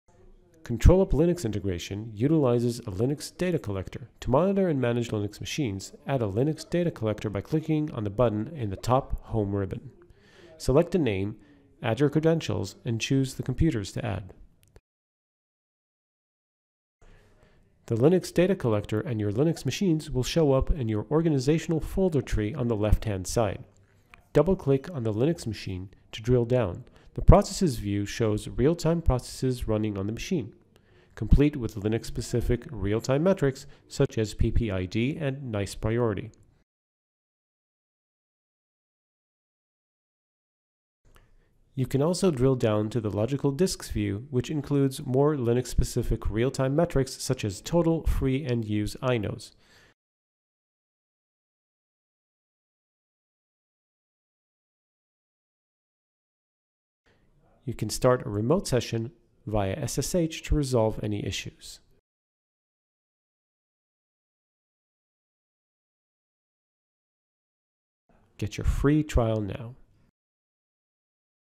ControlUp Learns Linux
See how ControlUp now integrates Linux machines so you can monitor, manage and troubleshoot your Linux VMs alongside your virtual infrastructure.